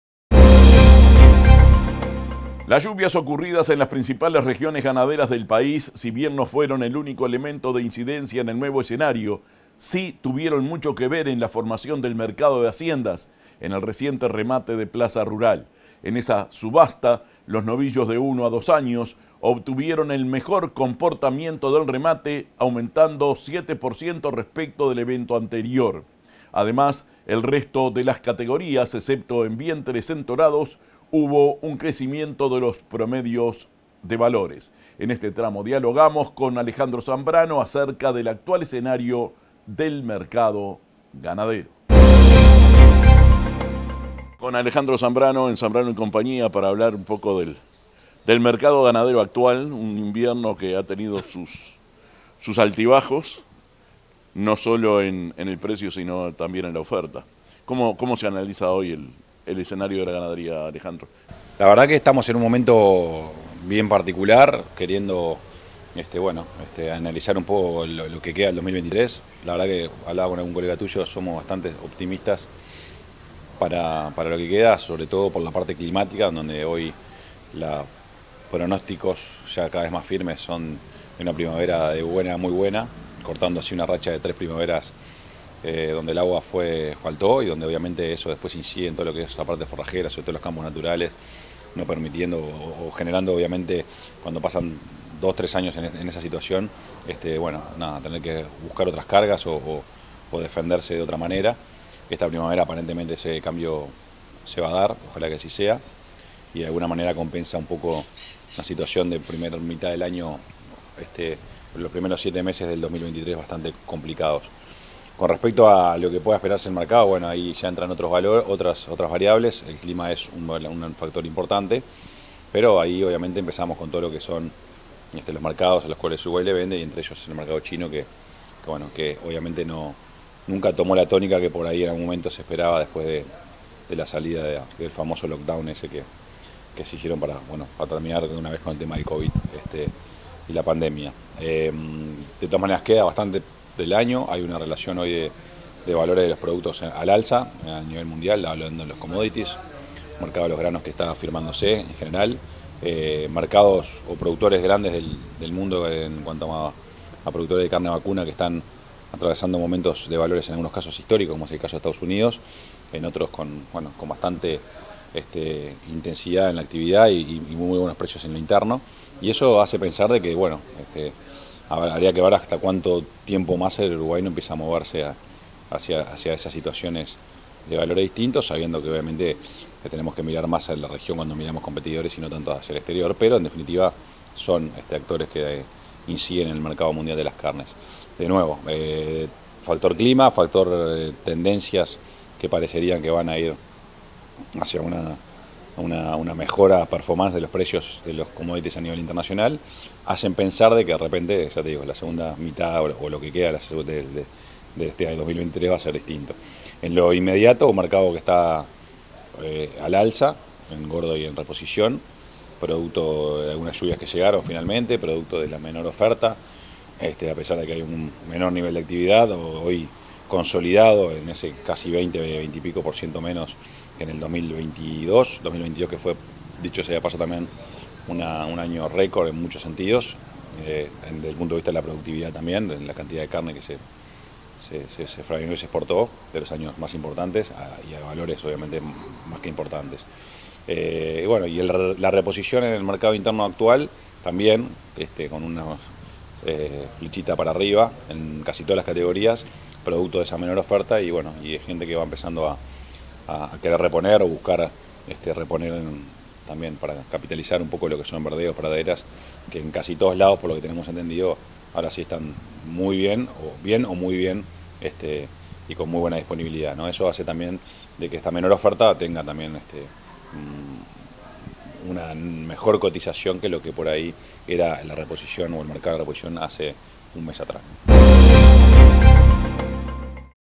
Alejandro Zambrano - Director de Zambrano &amp; Cía
Las lluvias ocurridas en las principales regiones ganaderas del país, si
bien no fueron el único elemento de incidencia en el nuevo escenario,
sí tuvieron mucho que ver en la formación del mercado de haciendas.
Dialogamos con Alejandro Zambrano, acerca del actual mercado
ganadero.